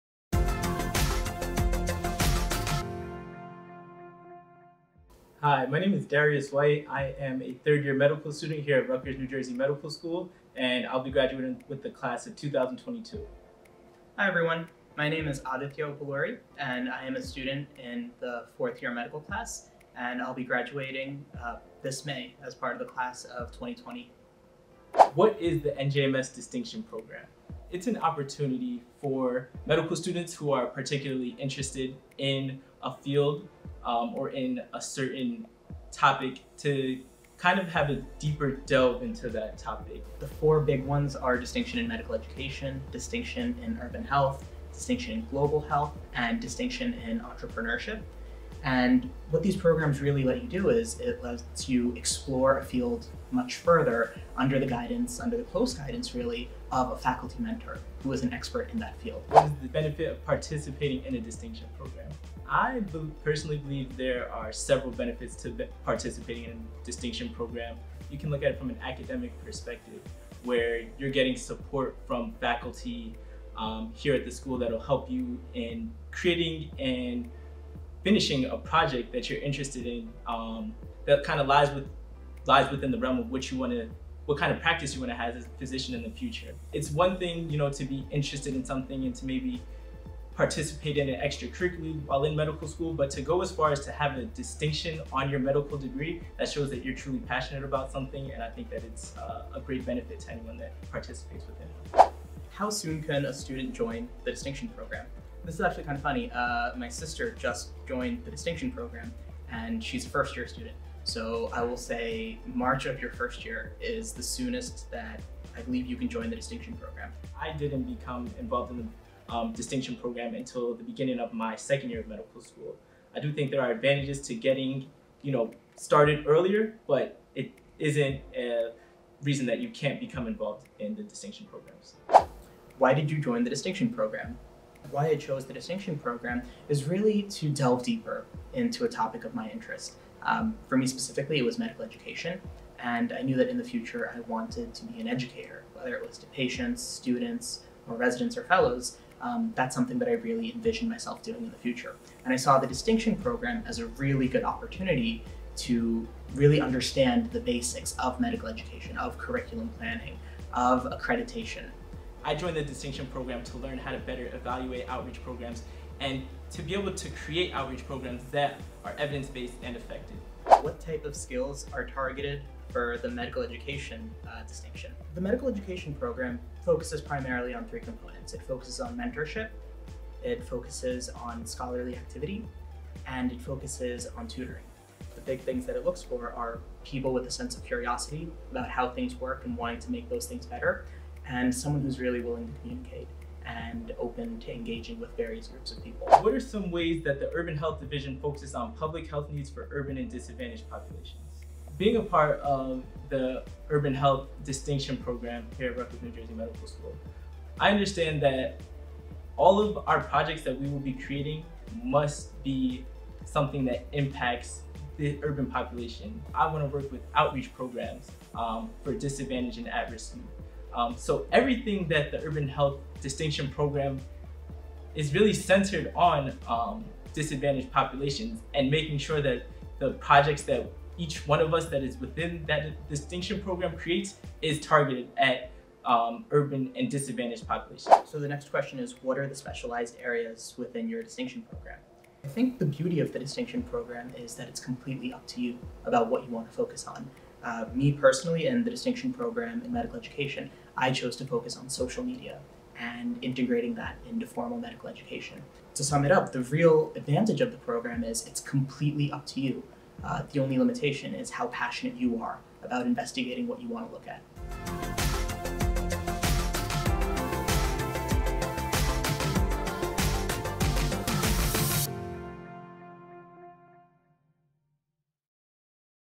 Inside the NJMS Distinction Program
NJMS third-year student Darius White and 2020 graduate Aditya Uppuluri discuss the school's Distinction Program.

Curious about the program? Get answers at the timestamps below.

Q: What is the NJMS Distinction Program? [0:25]
Q: What is the benefit of participating in a distinction program? [0:58]
Q: How soon can a student join the distinction program? [1:41]
Q: Why did you join the distinction program? [2:11]
Q: What type of skills are targeted for the medical education distinction? [2:49]
Q: What are some ways that the urban health division focuses on public health needs for urban and disadvantaged populations? [3:15]
Q: What are the specialized areas within your distinction program? [3:57]